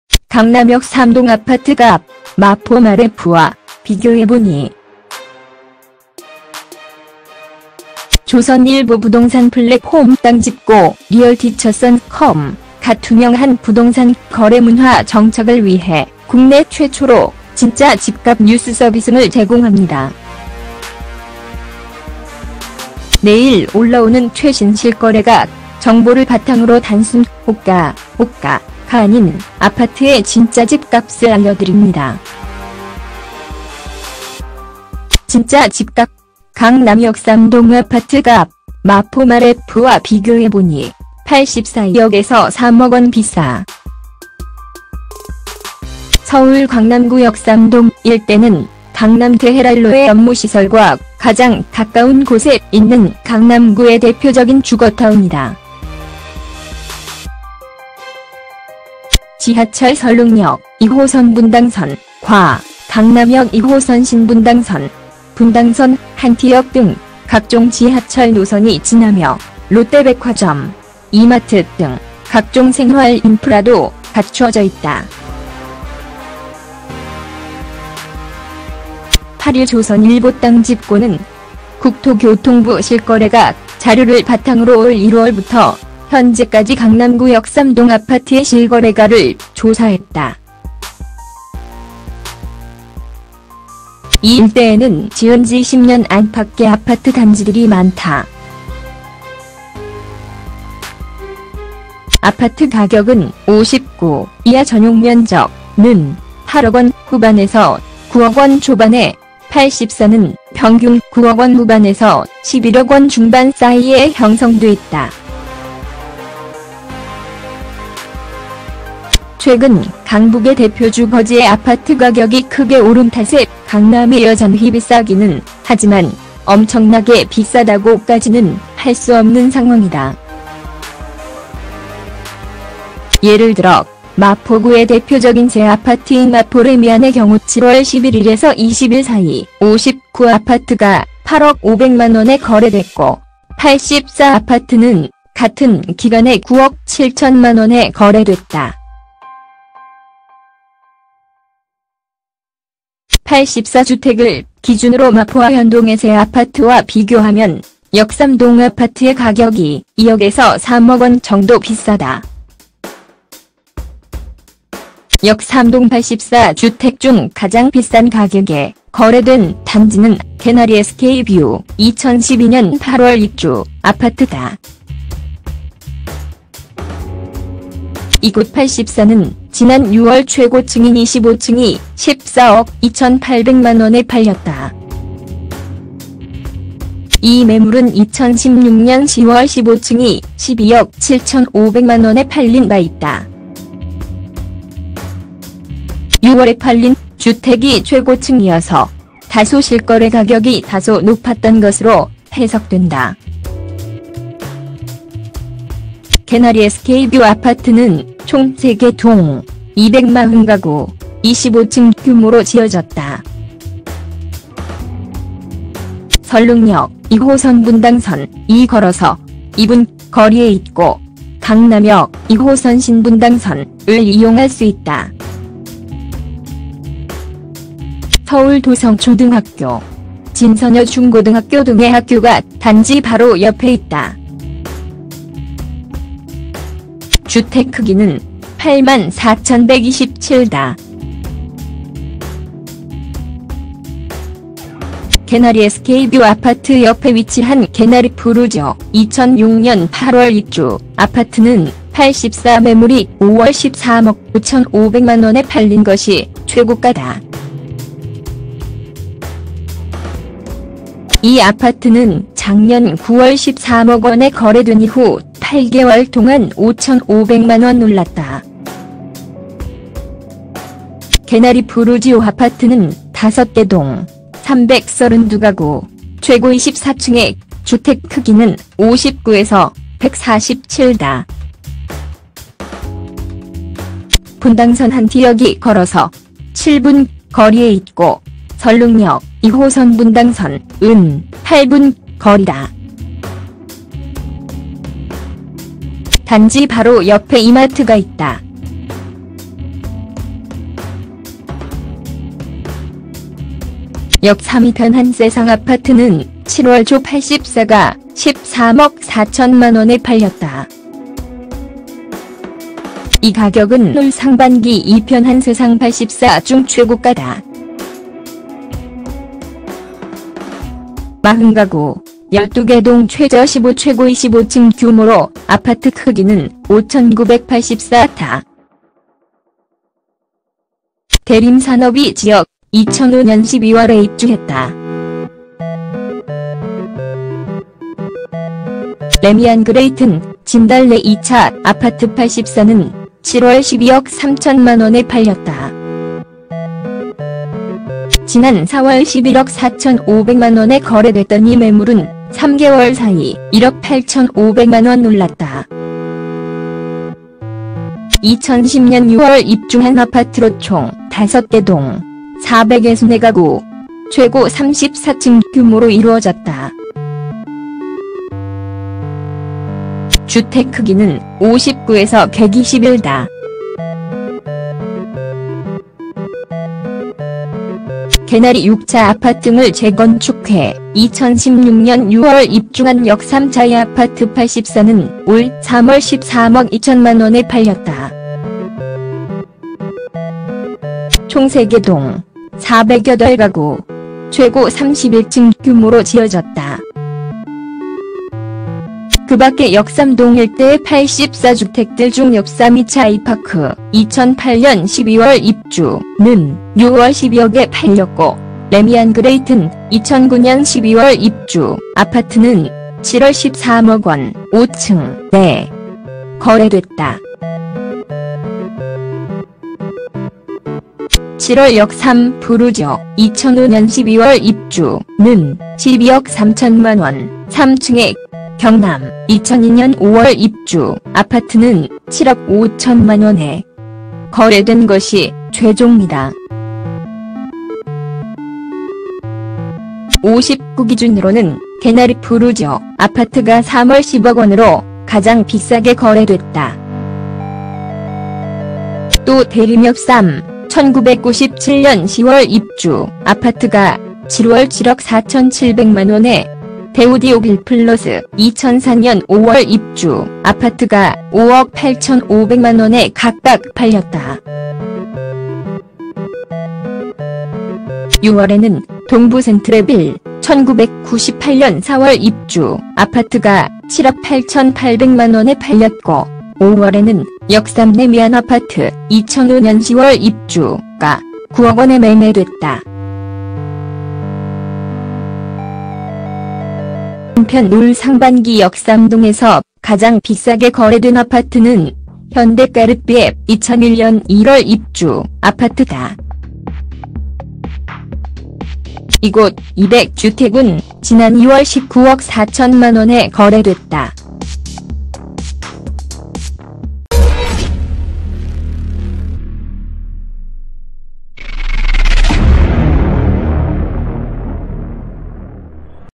강남 역삼동 아파트 값, 마포 마래푸와 비교해보니
신차뉴스, 자동차관리, 카라이프,BMW,X7,X8,HONDA,TOYOTA,KIAMO
BMW,X7,X8,HONDA,TOYOTA,KIAMO